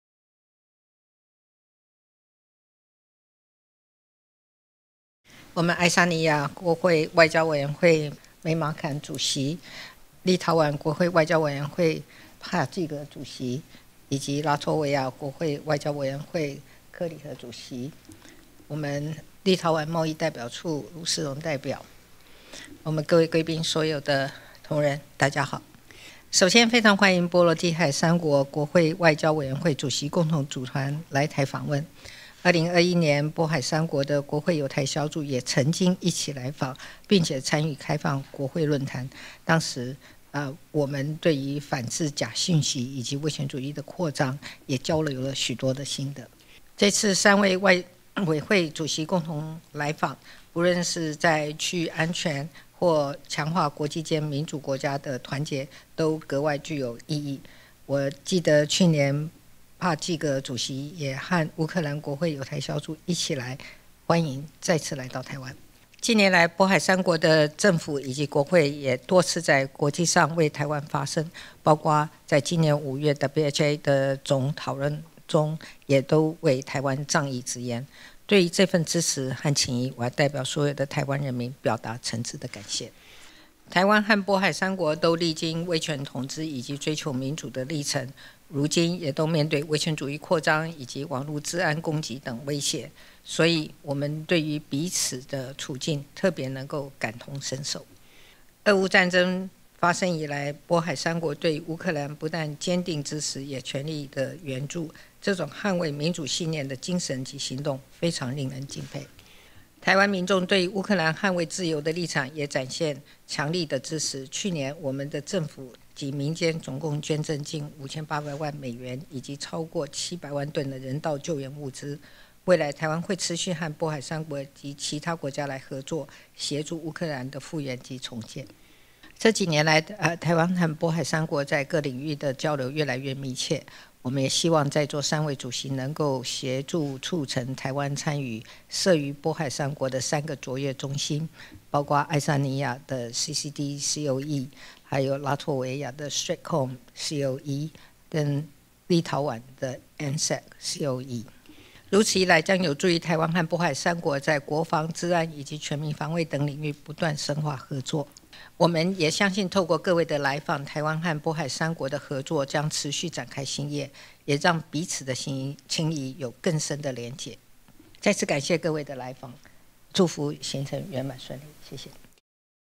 20230810 總統接見「波羅的海三國國會外交委員會主席訪問團」
蔡英文總統接見「波羅的海三國國會外交委員會主席訪問團」，感謝波海三國的政府及國會多次在國際上為臺灣發聲。並強調，這幾年來，臺灣和波海三國在各領域的交流，越來越密切。希望訪團協助促成臺灣參與設於波海三國3個「卓越中心」，讓臺灣和波海三國的合作持續展開新頁，也讓彼此情誼有更深的連結。